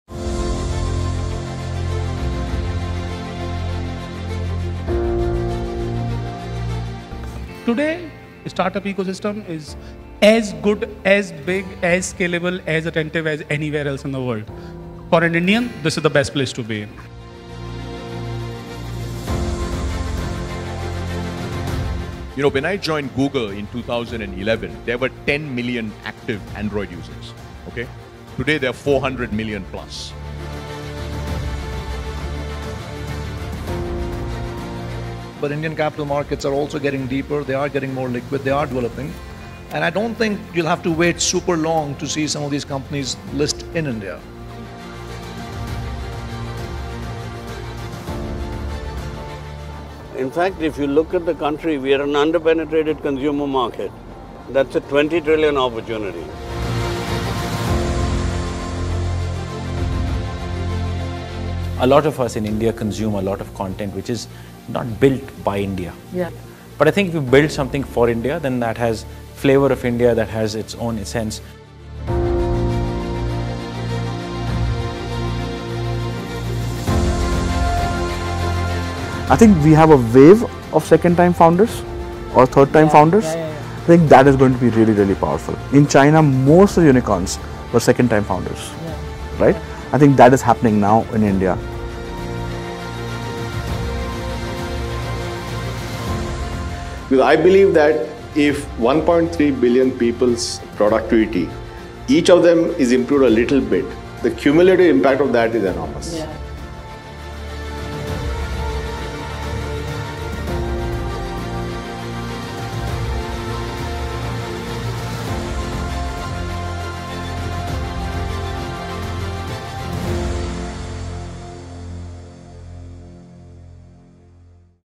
2019 - A year of the Great Indian opportunity
In this series of yearend videos, YourStory gives you a glimpse of what worked and what needs work in the Indian startup ecosystem.